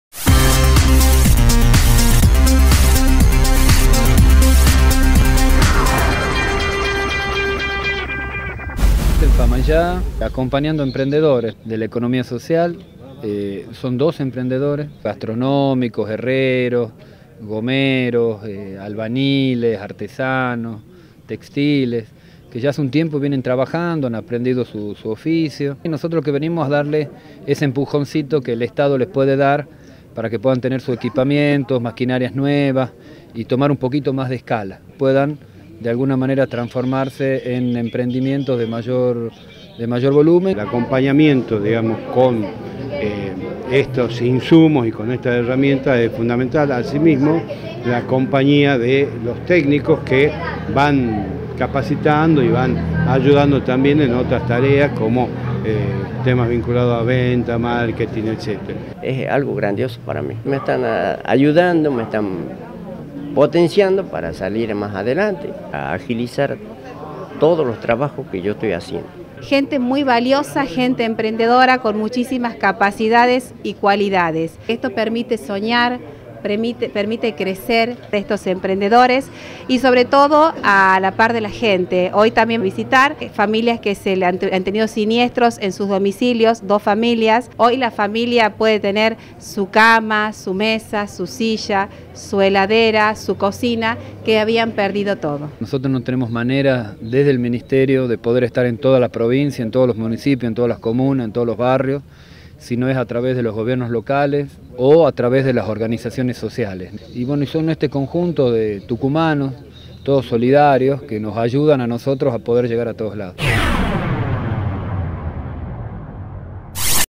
Entregaron insumos y herramientas a emprendedores de Famaillá - Tucumán Gobierno
Más de doce familias de Famaillá recibieron insumos y herramientas de trabajo que les permitirán potenciar el desarrollo de sus emprendimientos. La entrega, que se llevó a cabo en la réplica del Cabildo de esa ciudad, es el resultado del trabajo articulado entre la Dirección de Economía Social del Ministerio de Desarrollo Social y la oficina de Empleo de la Municipalidad.
El encuentro estuvo encabezado por el titular de la cartera Social, Gabriel Yedlin, y la intendenta del municipio, Patricia Lizárraga.
“Queríamos entregar personalmente herramientas y materiales que les permitirán a estos emprendedores y a sus familias a fortalecer su trabajo. Es un pequeño empujoncito que les sirve para potenciar sus fuentes de ingreso”, expresó Yedlin.
Por su parte, Lizárraga indicó que “es muy importante la presencia de Desarrollo Social en nuestra ciudad, ya que refleja la presencia y el acompañamiento permanente del gobierno y agregó que "con una pequeña ayuda, gente muy valiosa y emprendedora, ha podido salir adelante y sostener su propio trabajo”.
Por su parte,  el secretario de Articulación Territorial y Gestión Local, Ramiro González Navarro expresó: “Con esto, aspiramos a lograr una economía de complementariedad, que haga crecer el mercado interno. En este sentido, el acompañamiento del área es fundamental, no solo con la entrega de equipamiento, sino también con la capacitación permanente”.
José Bernardo Mansilla, herrero artístico, contó su experiencia: “Esta ayuda que nos ofrecen, brinda la satisfacción de hacer lo que nos gusta, con la posibilidad de salir adelante. Con lo que recibimos hoy, puedo agilizar mis trabajos, darles una mejor terminación. Me da mucha felicidad recibir estas herramientas e insumos, uno siente el acompañamiento del Estado”.